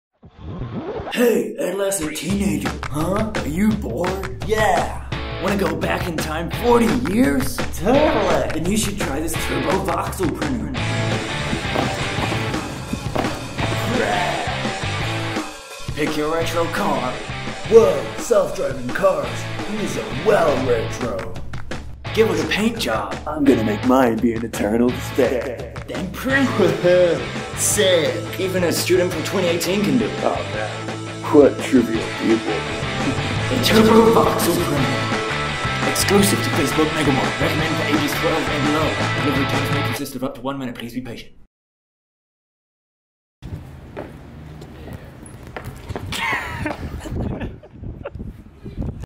The TURBO VOXEL Printer
A for-fun short film project with my voxel-printed car model.
Model and video created by Jordan Peat